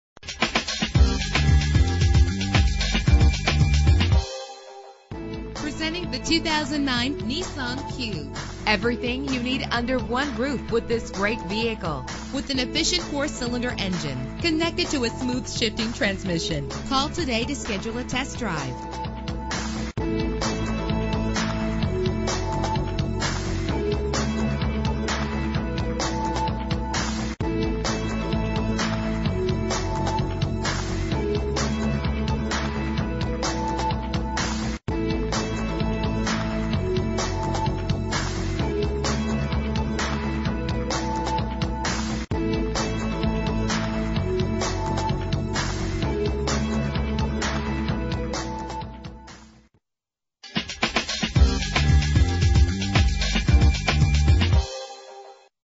2009 Nissan cube - Chesapeake VA
Year: 2009
Make: Nissan
Model: cube
Trim: 1
Engine: 1.8 liter inline 4 cylinder 16 valve
Transmission: Constant Velocity
Color: Scarlet Red
Mileage: 0
Address:
1717 S. Military Highway